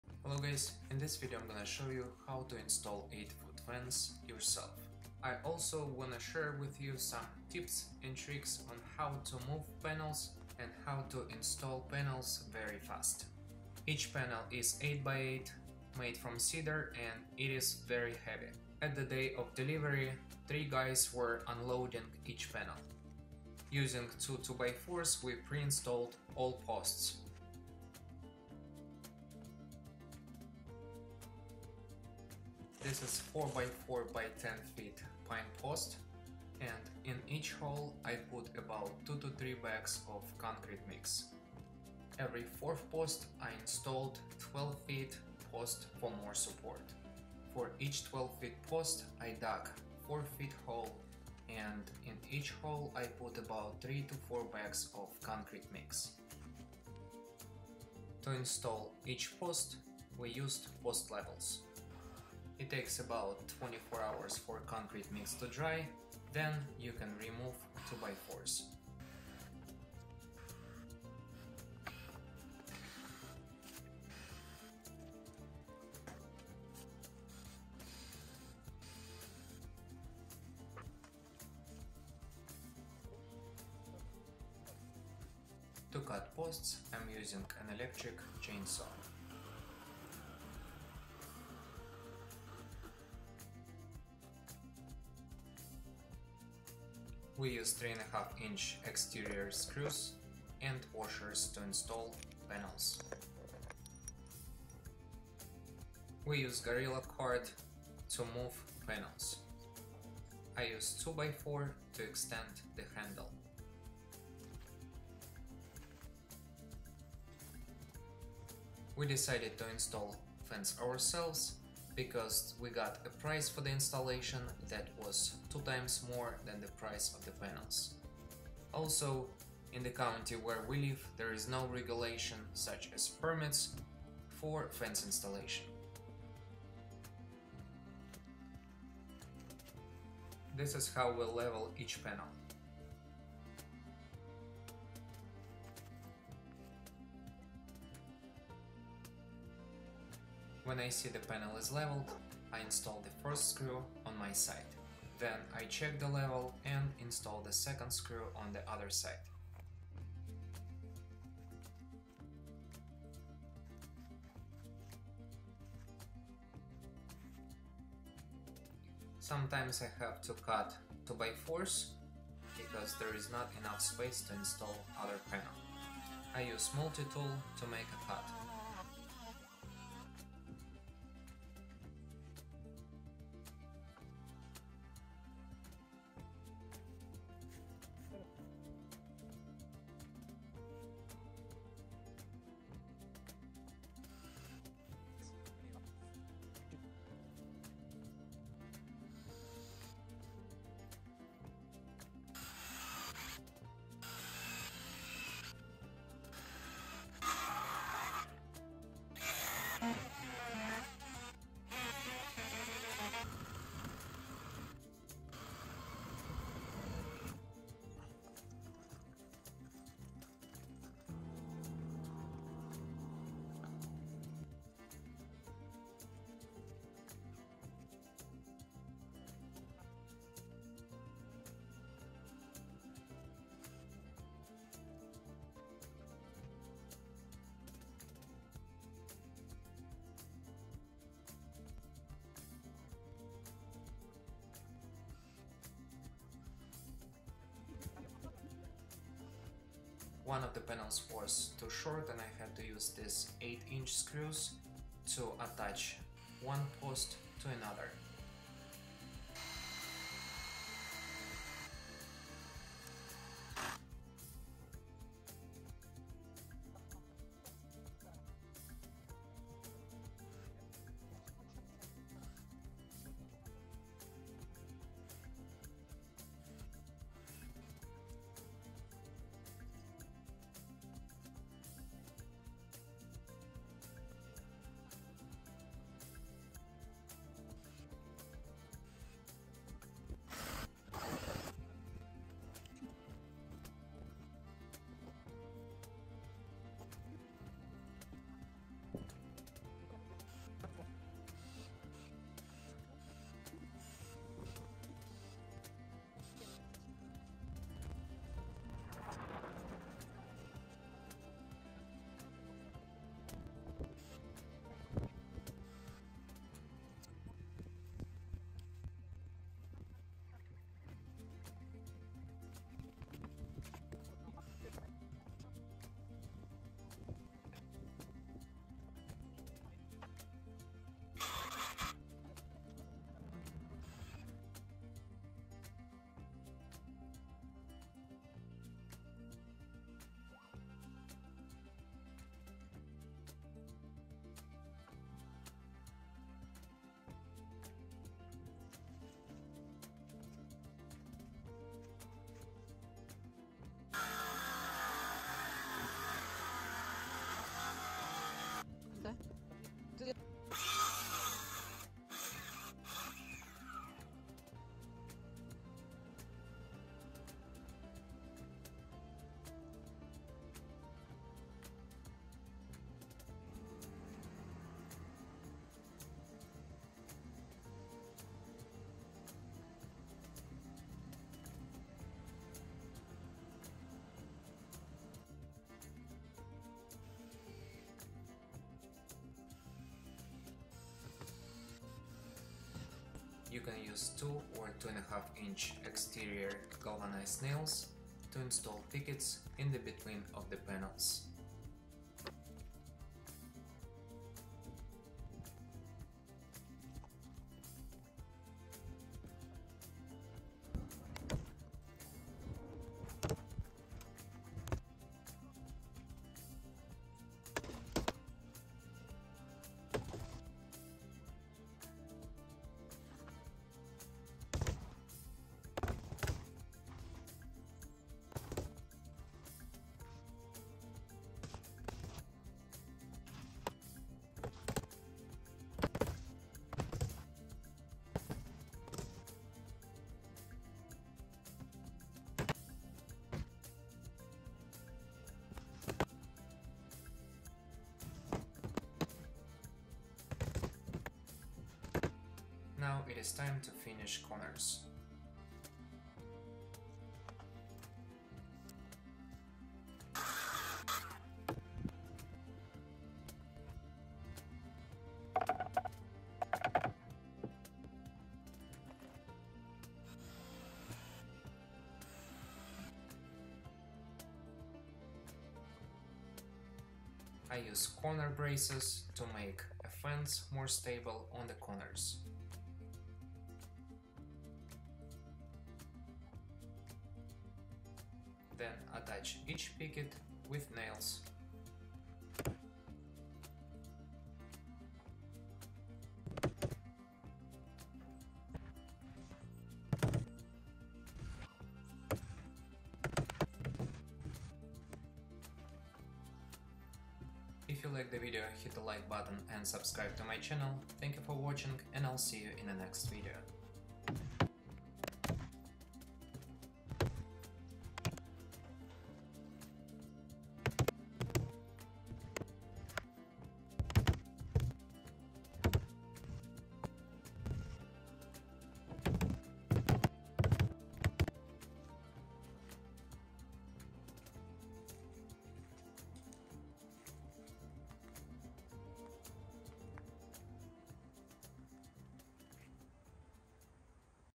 How to Install 8ft Wooden Fence
Hello guys, in this video I will show you how me and my wife installed 8 ft fence panels!

Products used in this video:

Utility (Gorilla Cart)
Electric Chainsaw
RIDGID Mixer
Ryobi Drill/Driver
3 1/2 Wood Screws
Ryobi 18V Multi Tool
Ryobi multi-tool blades
Tape Measure
Circular Saw
Level Tool, 24-Inch